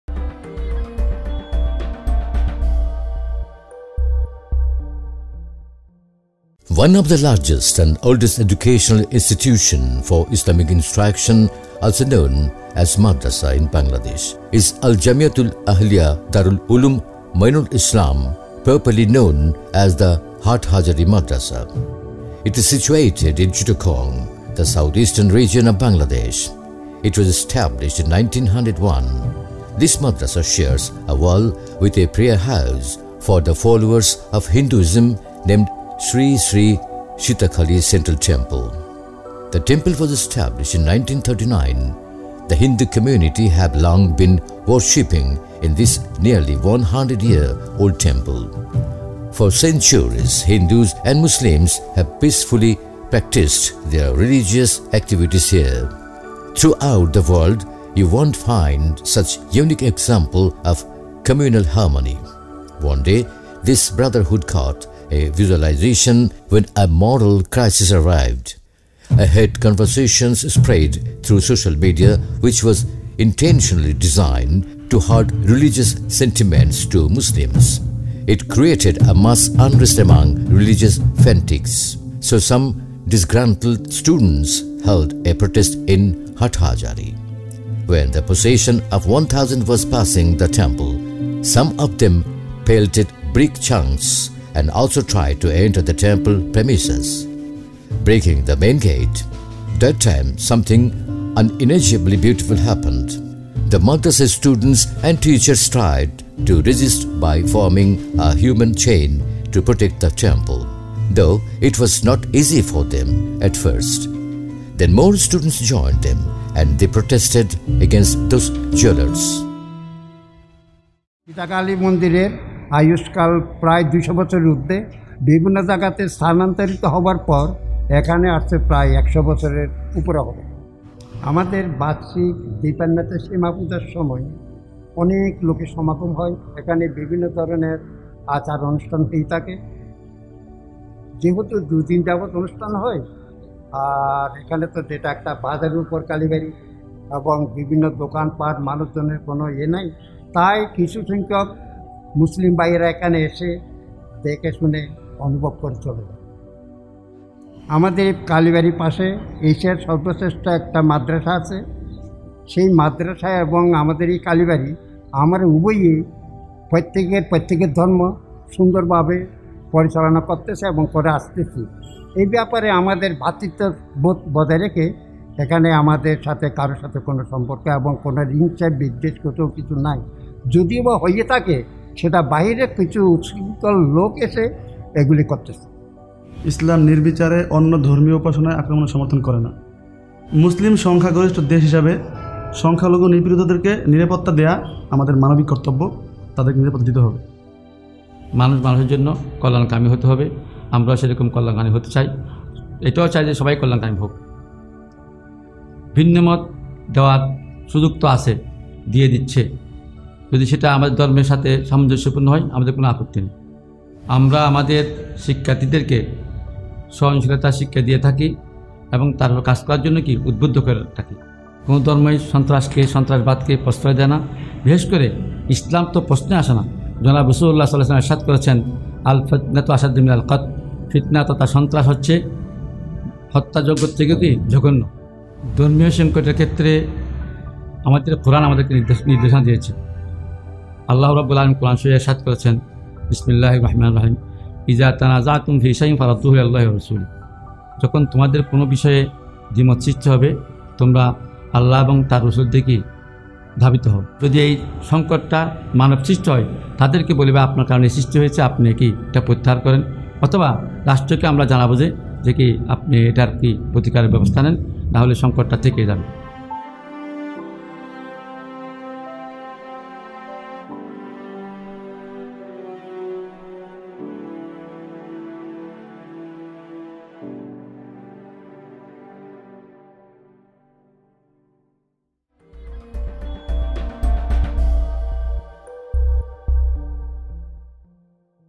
Islam and Hinduism in Bangladesh
The Sawt_Alhikma Center kicks off the Alhikma_films campaign with a documentary from Bangladesh about students of an Islamic school in Chittagong who formed a human chain to protect the Hindu temple adjacent to their school from attacks.